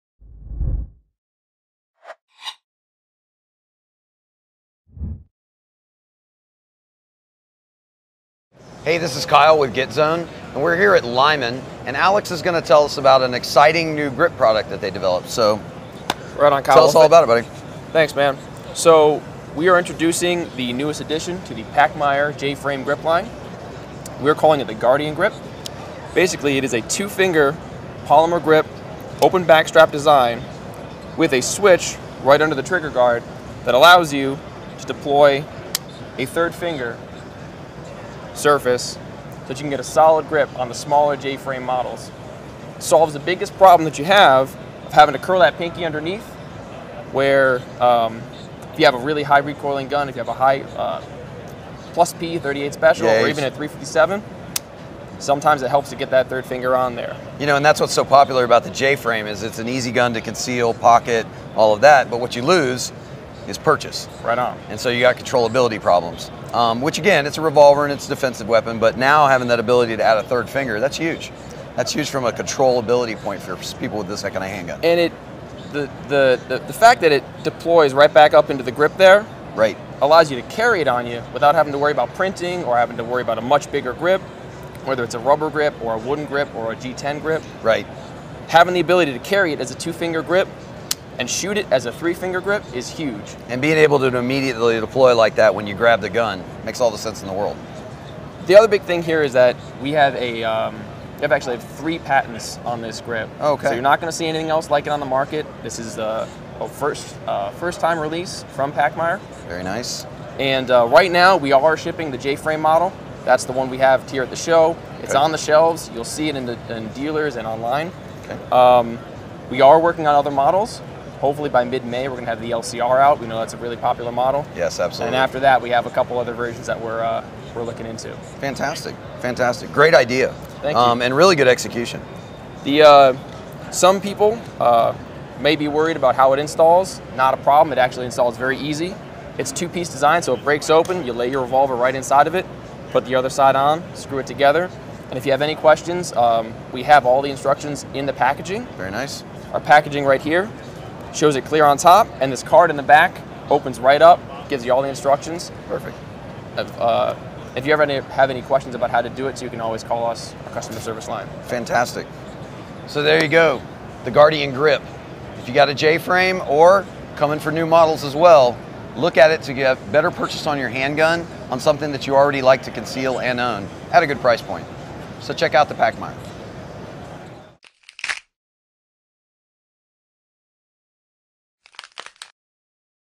2018 New Gear and Guns: Lyman Introduces the New Pachmayr GuardianGrip
Pachmayr’s new GuardianGrip is a revolutionary and innovative design made for popular concealed carry revolvers. What makes the GuardianGrip so unique is its patented finger extension. With the simple press of a button, the spring loaded finger extension drops down out of the grip, converting a two finger compact grip into a controllable three finger grip. This is the first grip which allows the concealment of a compact grip with the shooting control of a full size grip.